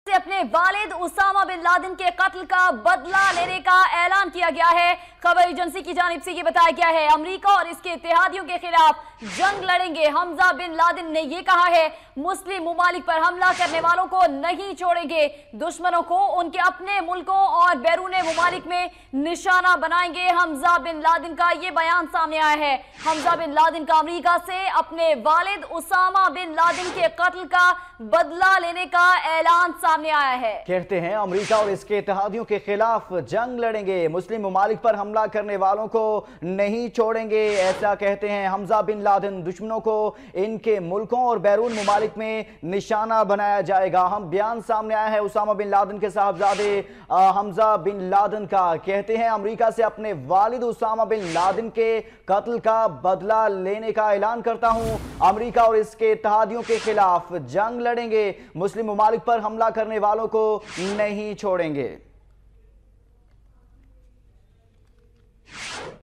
حمزہ بن لادن کا امریکا سے اپنے والد اسامہ بن لادن کے قتل کابدلہ لینےکااعلان خبرایجنسی YouTube
Breaking News America Barbad Hony Wala Hy